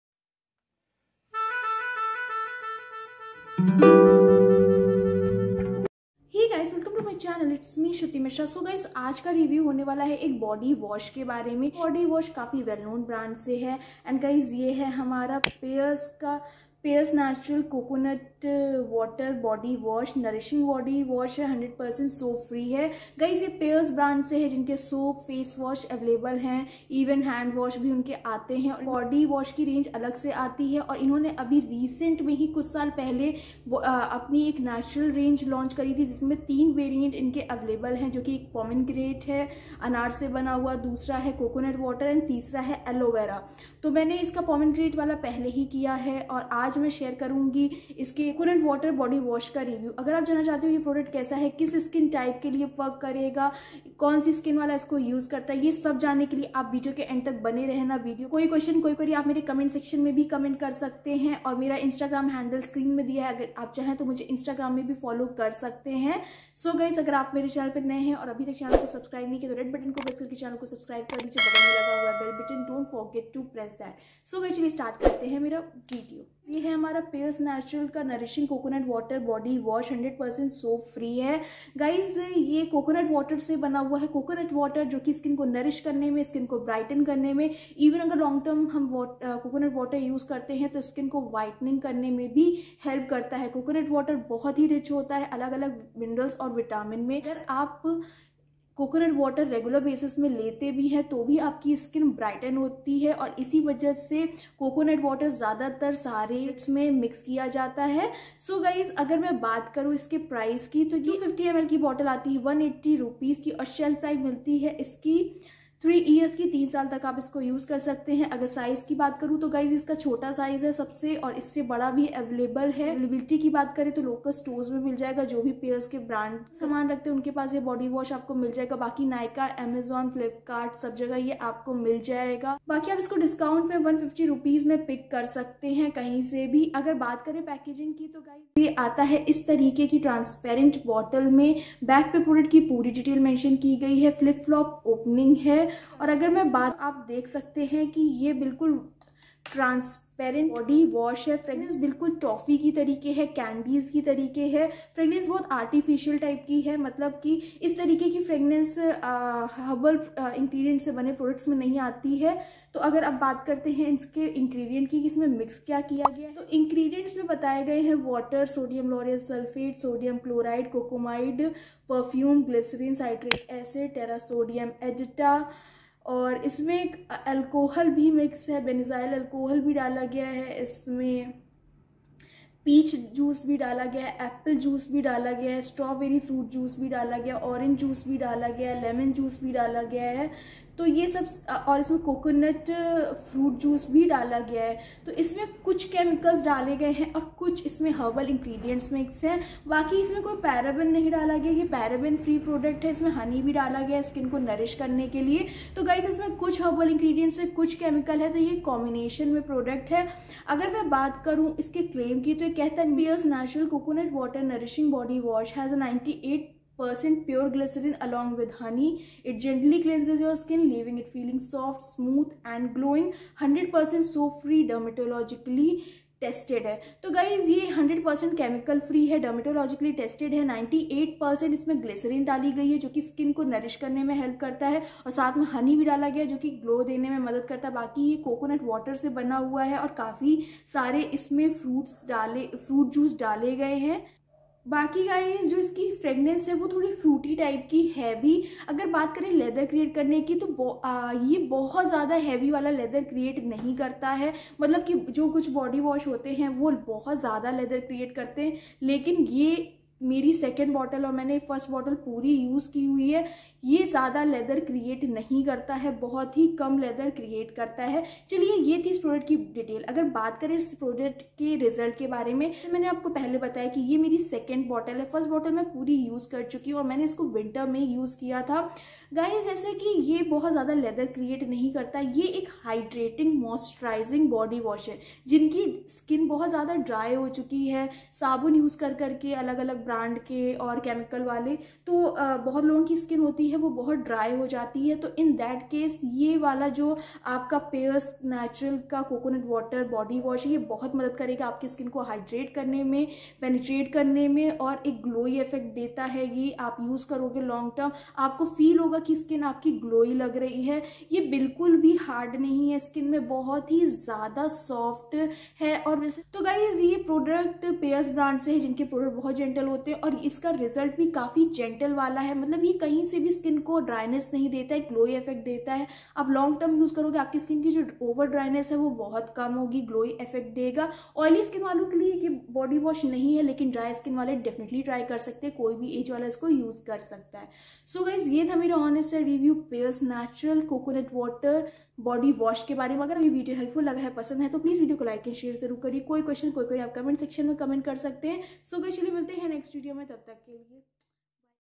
Full Body Whitening..?| Pears Naturale Coconut Water Body Wash | फायदे और नुकसान | Shruti Mishra
My Video is on..Full Body Whitening..? Pears Naturale Coconut Water Body Wash फायदे और नुकसान

Shruti Mishra
Xoxo

Shruti Mishra
ShrutiMishra